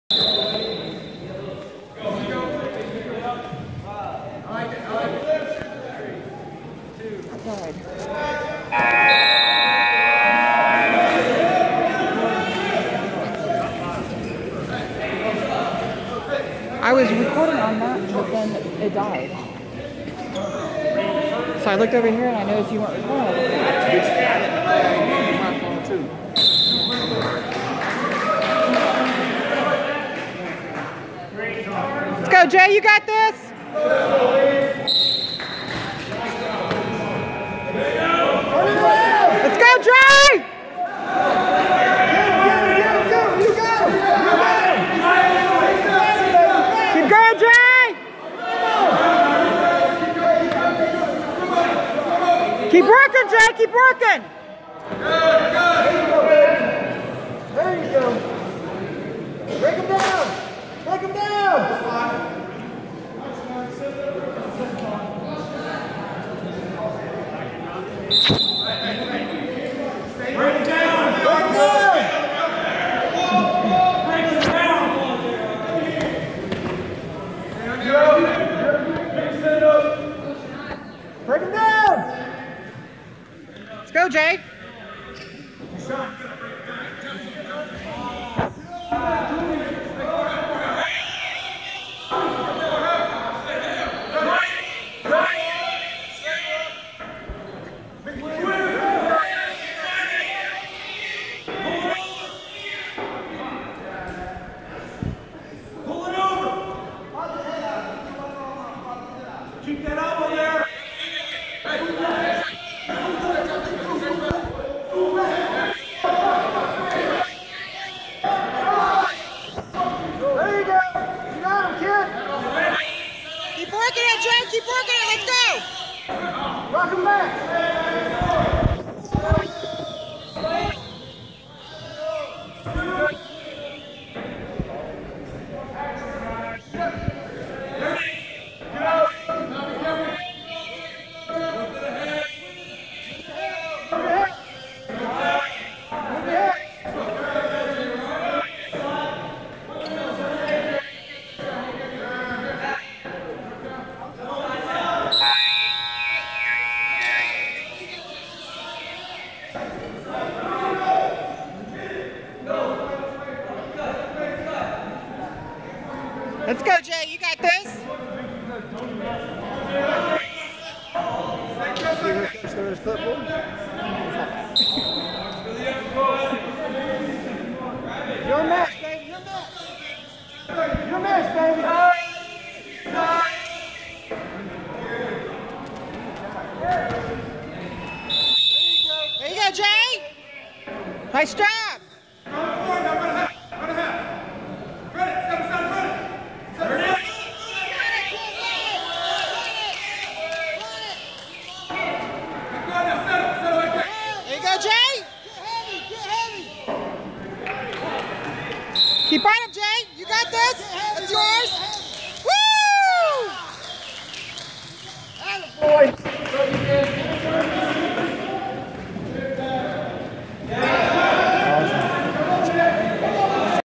J'shon Kekoa Horn: C/O 2020 (220 lb.)
J'shon Kekoa Horn: C/O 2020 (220 lb.) at South Aiken High School on Dec. 15, 2018 vs. Battle Creek Academy.